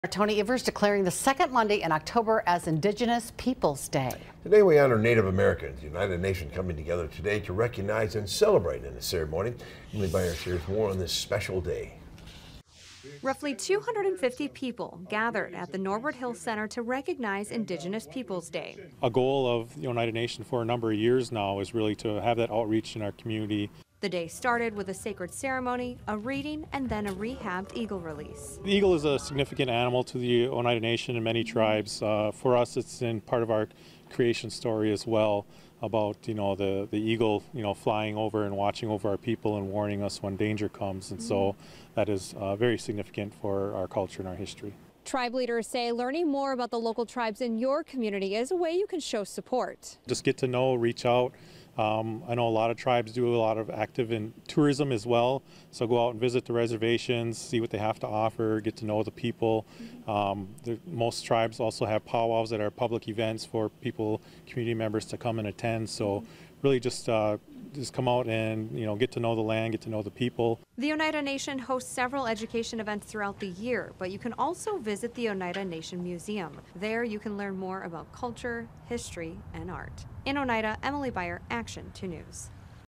Oneida community celebrates Indigenous Peoples Day
The Oneida Nation came together Monday to recognize and celebrate in a ceremony.